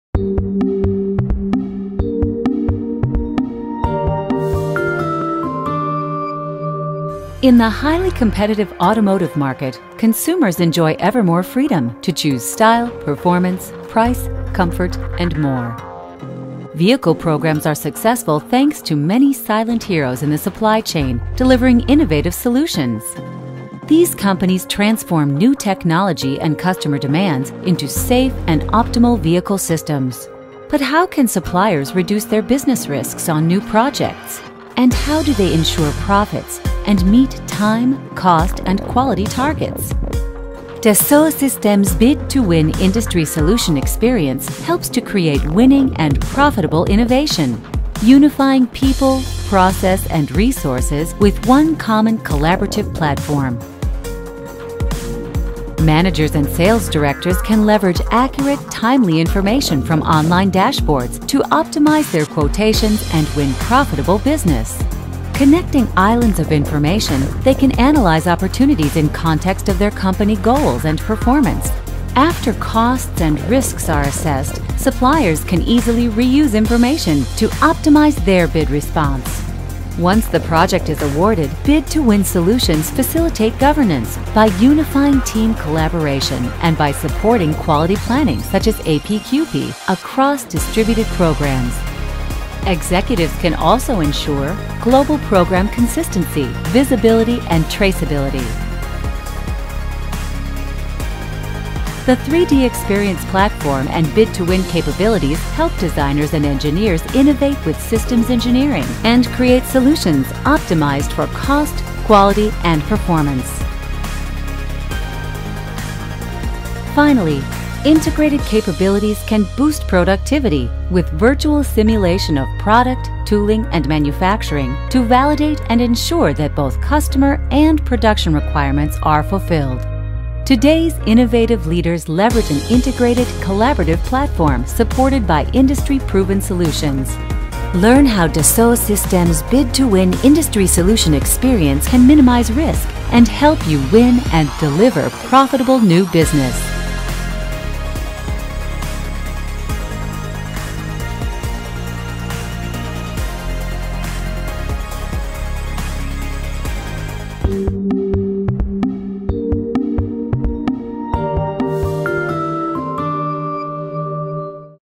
Get your global automotive supplier advantage - Dassault Systèmes
Get your global automotive supplier advantage - Dassault Systèmes. Explore how you and your supplier teams can win new, profitable opportunities, design and engineer to value, and deliver on target for automotive OEM requirements.